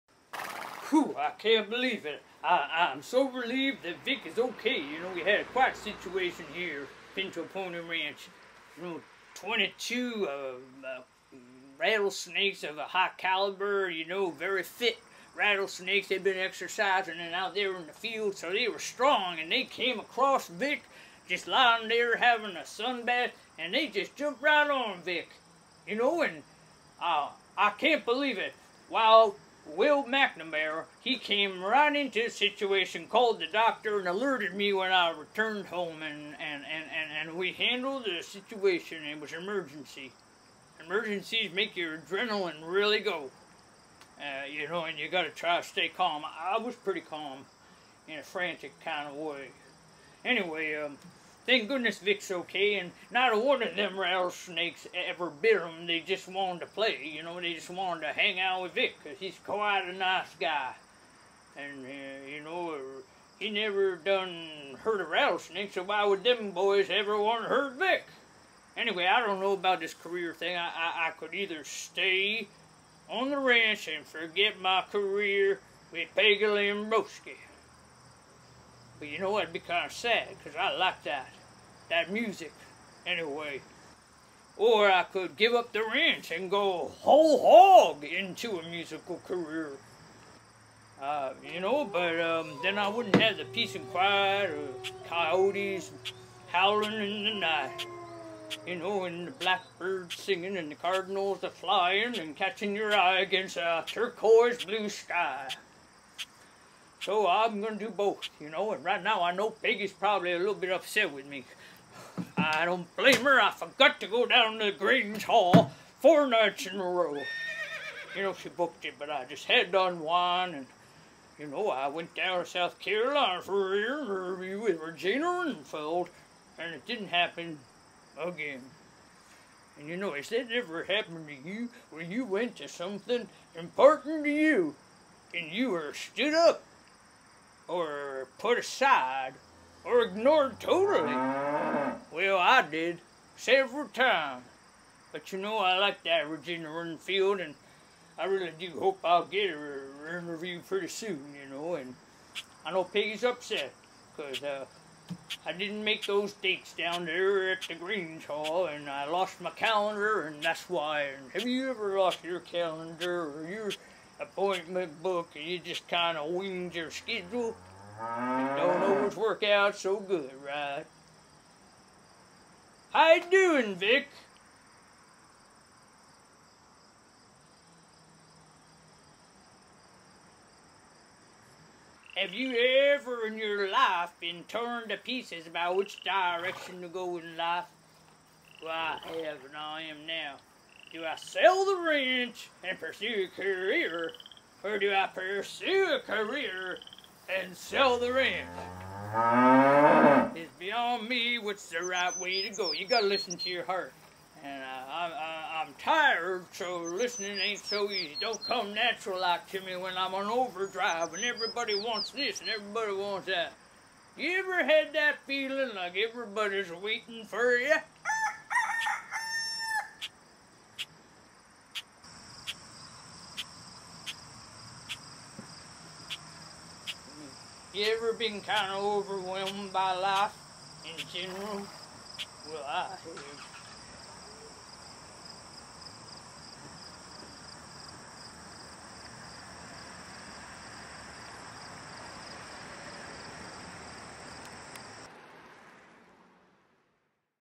Episode 34: Ponderings
Jesse Ray contemplates many profound things in his complicated life.